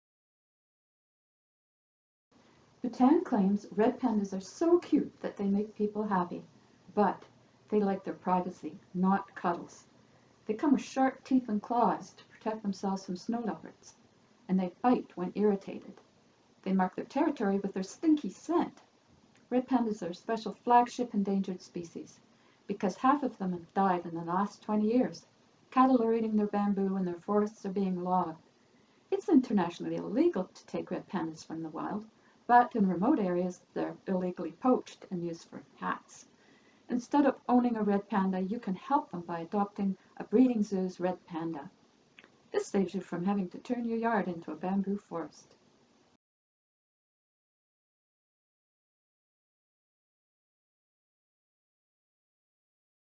Red Pandas - Cute But Not Cuddly * Don't Make Good Pets *
Red Pandas are one of the cutest animals on the planet.  They are a threatened species dependent on the bamboo forests of the Himalayas for their survival.
for information on how you can help save this charismatic species.

The Red Panda is a unique vegetarian carnivore that feeds almost entirely on bamboo.  For more information on the biology of the Red Panda,